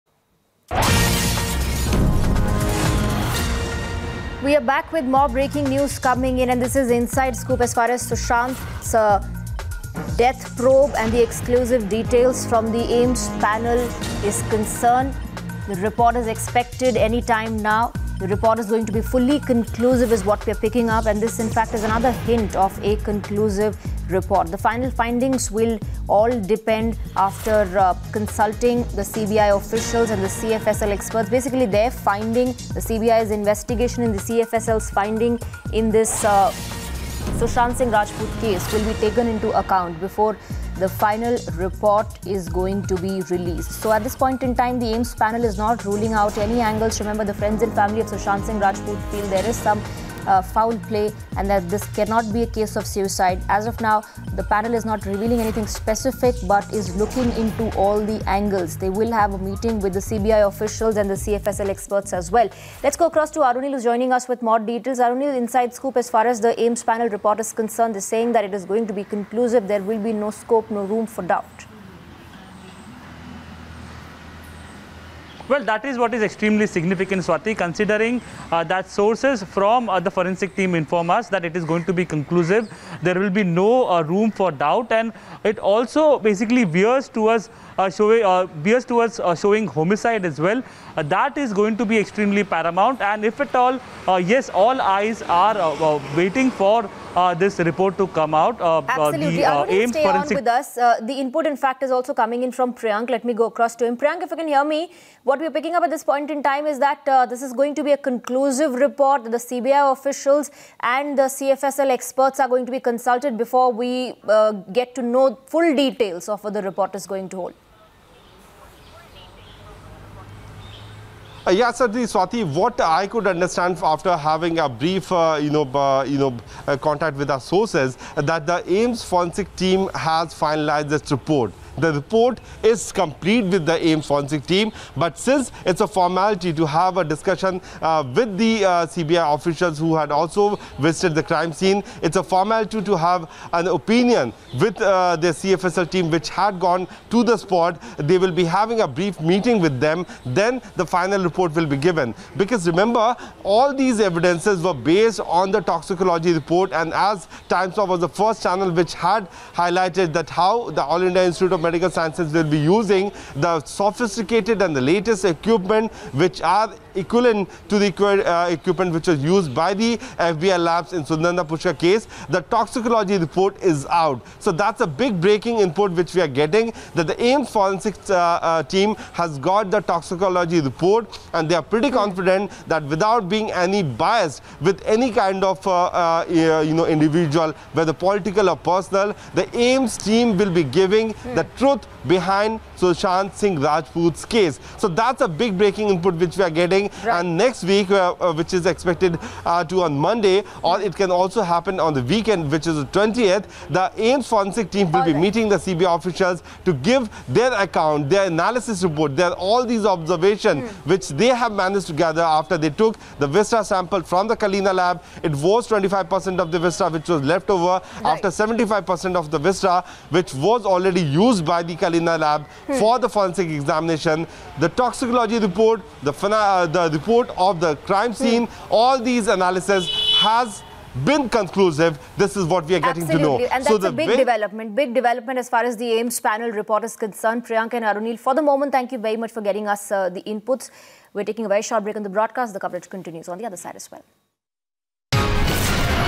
SSR Death probe: AIIMS report's EXCLUSIVE details accessed; Another hint of homicide exposed
A piece of shocking comes to light in the Sushant Singh Rajput’s death investigation. Times Now accesses inside scoop and exclusive details from the AIIMS panel. The postmortem report of Sushant conducted by the All India Institute of Medical Science lays out another hint or suicide.

Three parallel probes are being conducted in the case, however, CBI is particularly investigation the death mystery. CBI has previously stated that the new post-mortem report is completely conclusive and has no confusions. The panel is not revealing anything, but CBI ensured that no angle will be ruled out. CBI and AIIMS will have a meeting soon.